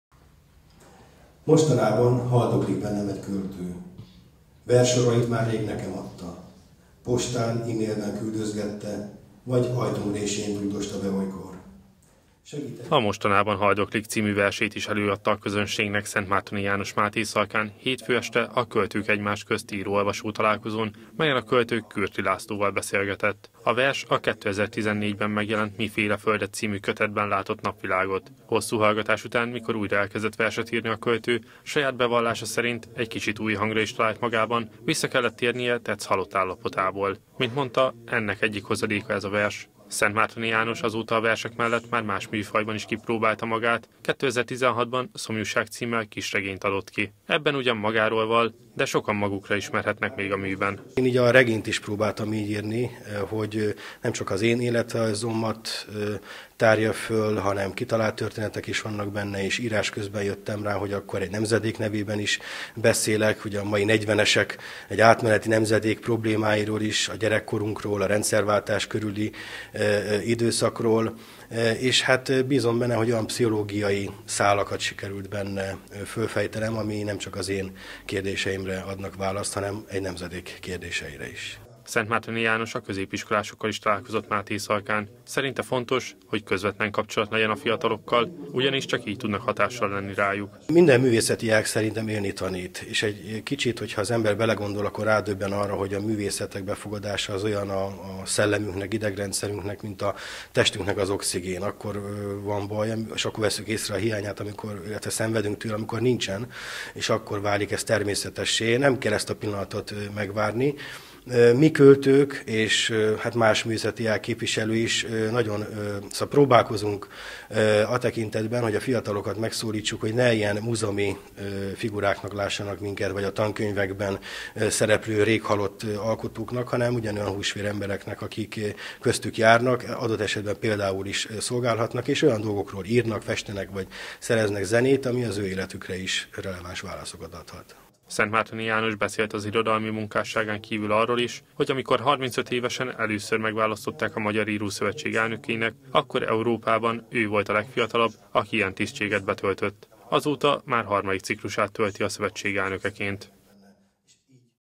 Költők egymás közt - Kölcsey Tv
Szentmártoni Jánossal beszélgetett Kürti László a Költők egymás közt író-olvasó találkozón hétfő este. A Mátészalkán rendezett irodalmi esten az író költő vallott költészetéről, a műfajváltásáról, de arról is, hogy mit jelent számára az, hogy már harmadik ciklusát tölti a Magyar Írószövetség elnökeként.